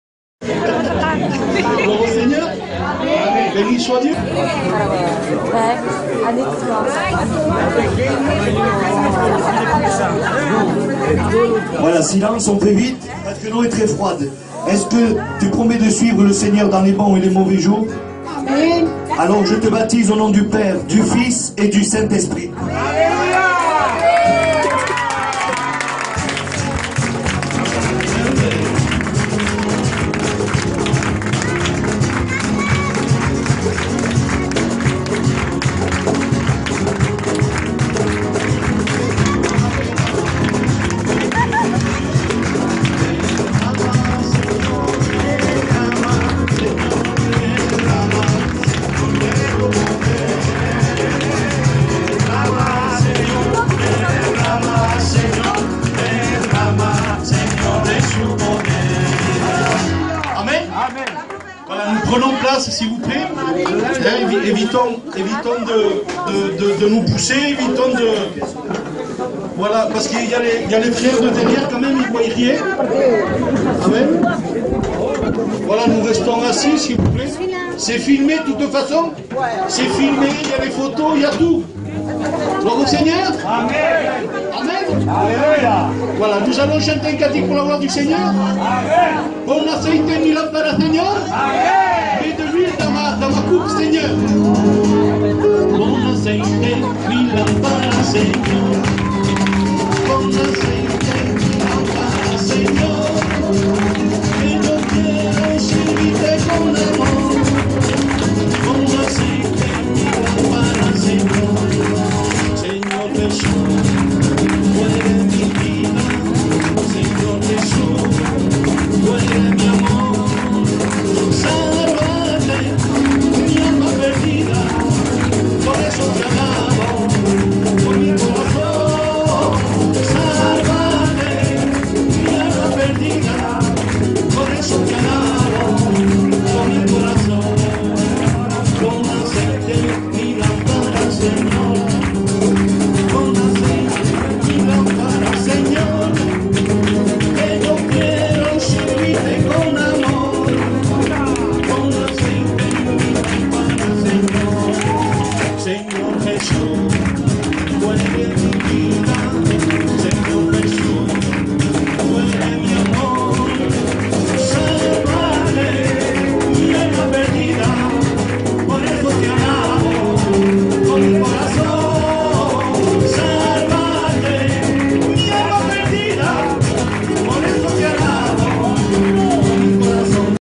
343. Baptême de notre sœur Sophia de Nice.
Mathieu 28-19:
Allez, faites de toutes les nations des disciples, les baptisant au nom du Père, du Fils et du Saint-Esprit,